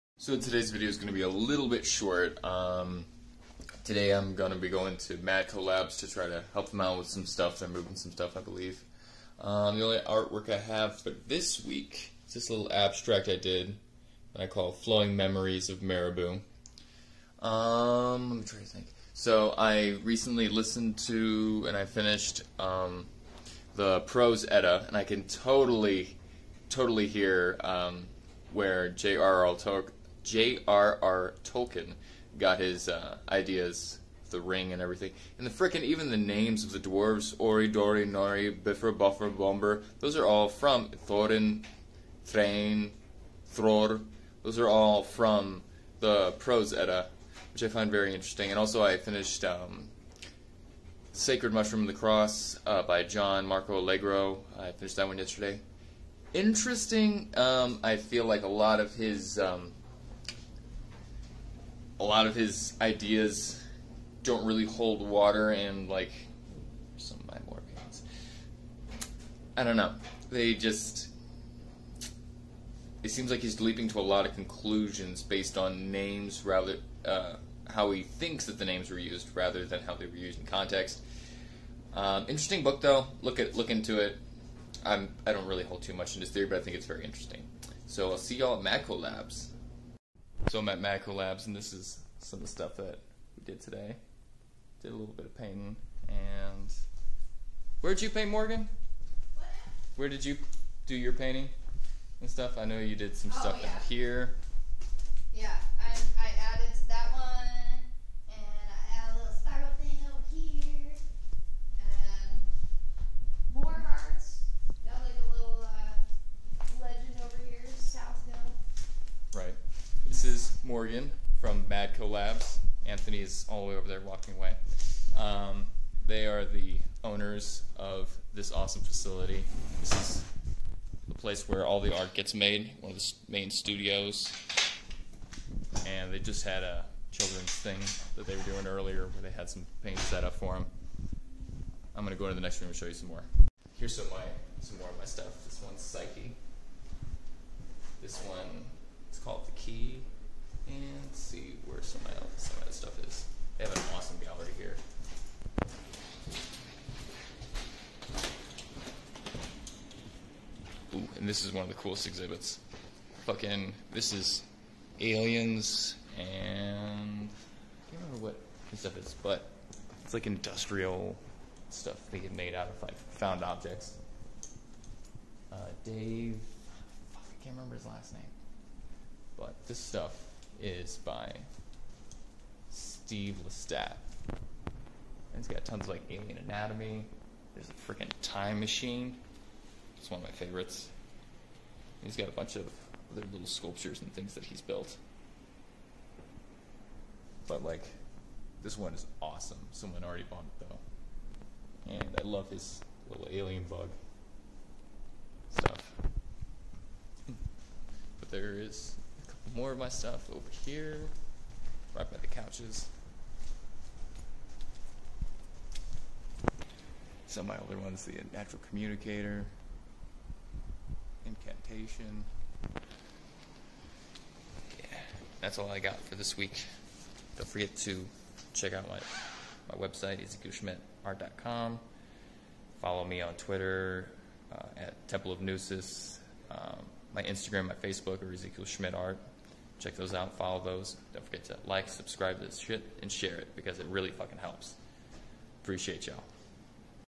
EYDS Vlog #8 Sacred Mushroom and the Cross/Prose Edda / Tour through MadCoLabs Studios!
today is a special treat where y'all get to see where some of my art hangs as well as meet Morgan one of the owners of Madcolab Studios in Spokane Wa!

facebook meme page @dankmemestoconfuseyou

join our Self Educators Book Club on discord!